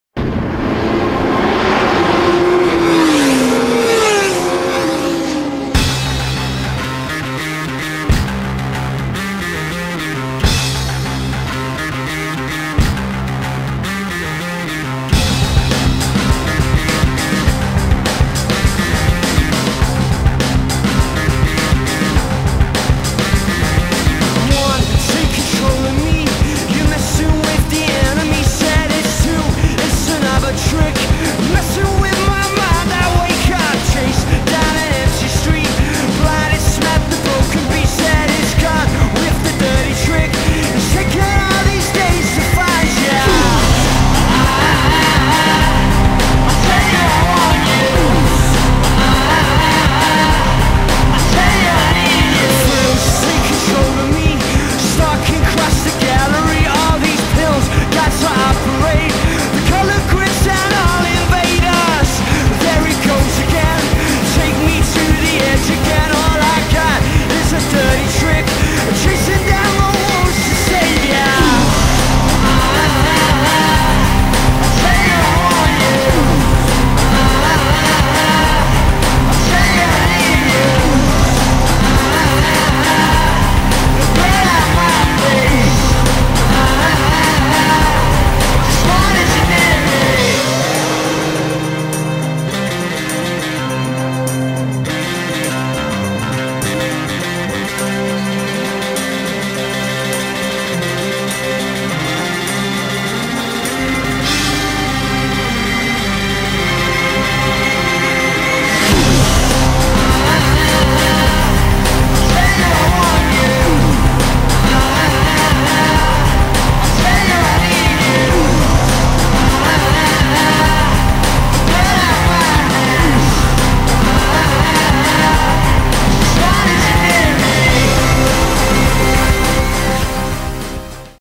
r1 at donnington race track bike crash
chap fell off his bike at donnington race track. Caught on helmet camera using a dogcam bullet camera system